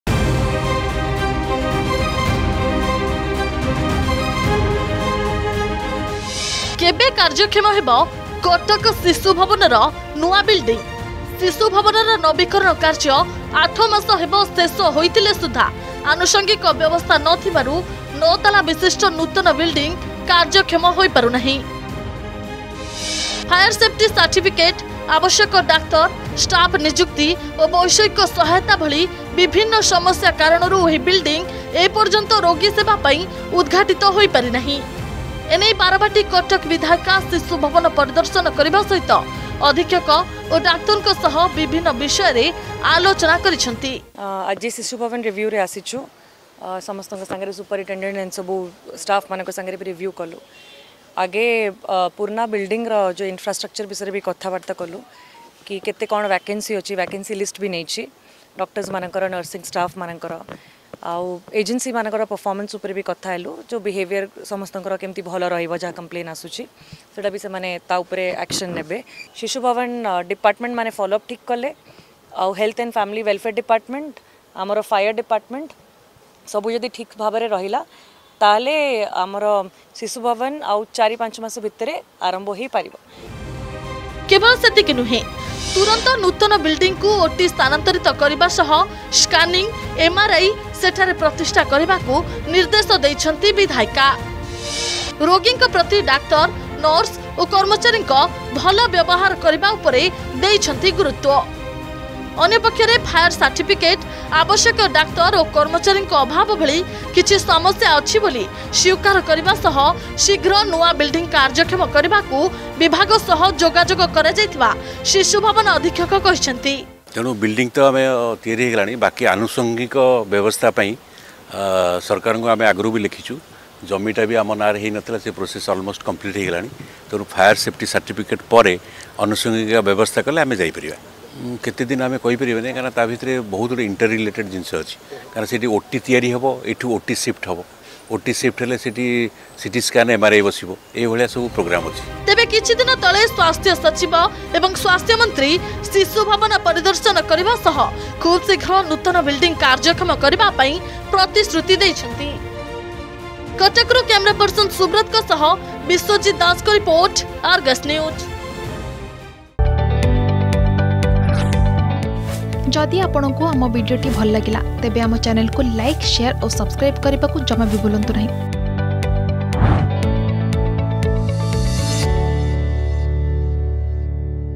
କେବେ କାର୍ଯ୍ୟକ୍ଷମ ହେବ କଟକ ଶିଶୁଭବନର ନୂଆ ବିଲ୍ଡିଂ ? || Cuttack Sishu Bhawan Issue
କେବେ କାର୍ଯ୍ୟକ୍ଷମ ହେବ କଟକ ଶିଶୁଭବନର ନୂଆ ବିଲ୍ଡିଂ ? । ଶିଶୁଭବନର ନବୀକରଣ କାର୍ଯ୍ୟ ଆଠ ମାସ ହେବ ଶେଷ ହୋଇଥିଲେ ସୁଦ୍ଧା ଆନୁଷଙ୍ଗିକ ବ୍ୟବସ୍ଥା ନଥିବାରୁ 9 ତାଲା ବିଶିଷ୍ଟ ନୂତନ ବିଲ୍ଡିଂ କାର୍ଯ୍ୟକ୍ଷମ ହୋଇ ପାରୁନାହିଁ । ଏନେଇ ବାରବାଟୀ-କଟକ ବିଧାୟିକା ଶିଶୁଭବନ ପରିଦର୍ଶନ କରିବା ସହିତ ଅଧୀକ୍ଷକ ଓ ଡାକ୍ତରଙ୍କ ସହ ବିଭିନ୍ନ ବିଷୟରେ ଆଲୋଚନା କରିଛନ୍ତି ।